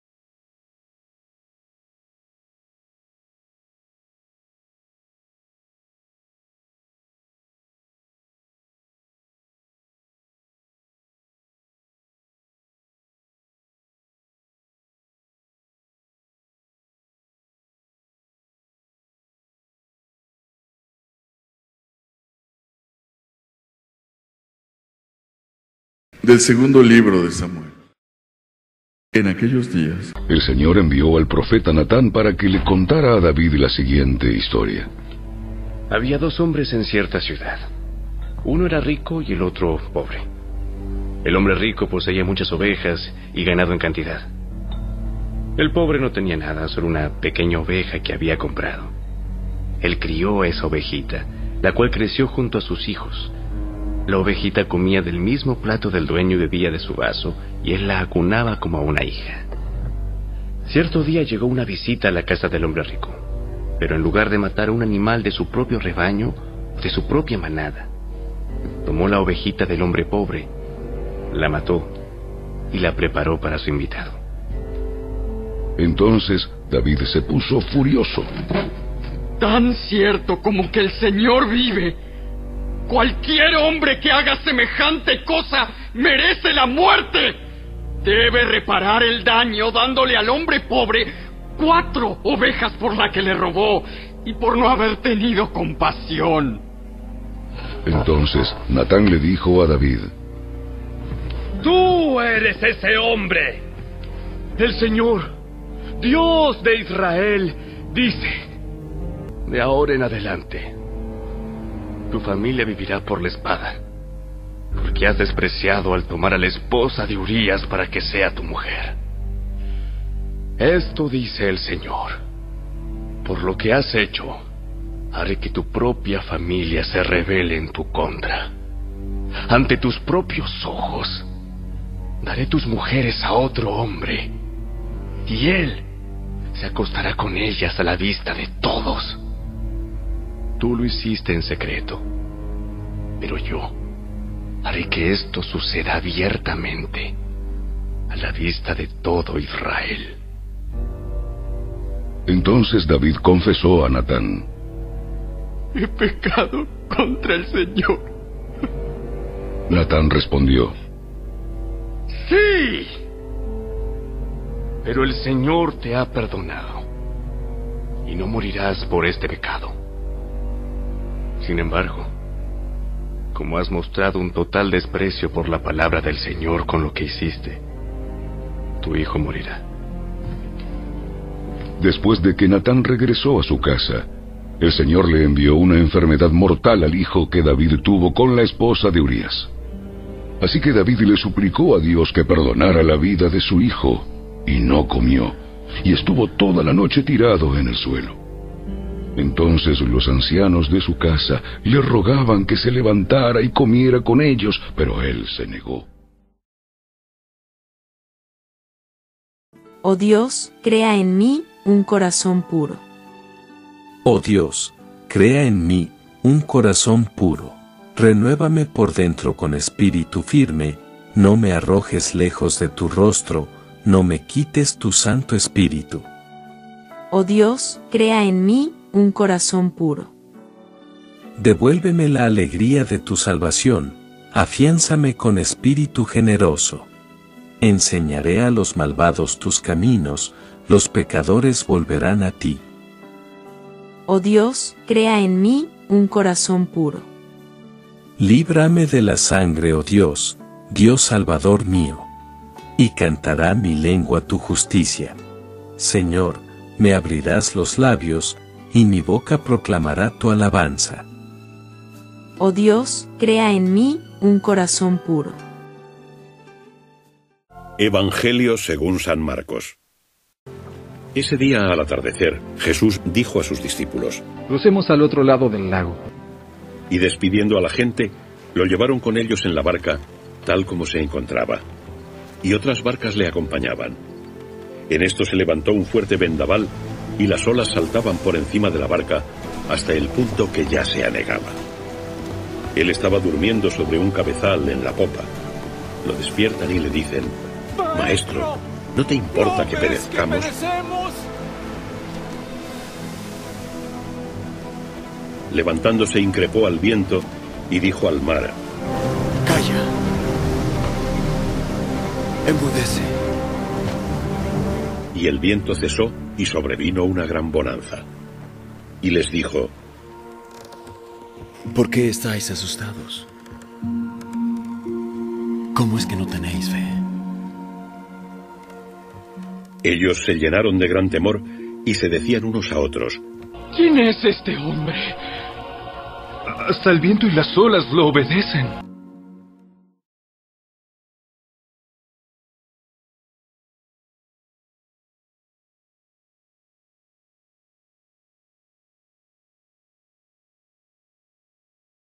EVANGELIO SÁBADO 27 DE ENERO DE 2024 | MARCOS 4,35-41 | SÁBADO 3° TIEMPO ORDINARIO.
EVANGELIO Y LECTURAS EUCARISTÍA SÁBADO 27 DE ENERO DE 2024 SÁBADO TERCERA SEMANA DEL TIEMPO ORDINARIO CICLO B. Primera Lectura: Del segundo libro de Samuel 2Sm 12,1-7a.10-17 Salmo 50,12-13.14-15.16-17 R/. Oh Dios, crea en mí un corazón puro Del Santo Evangelio según san Marcos 4, 35-41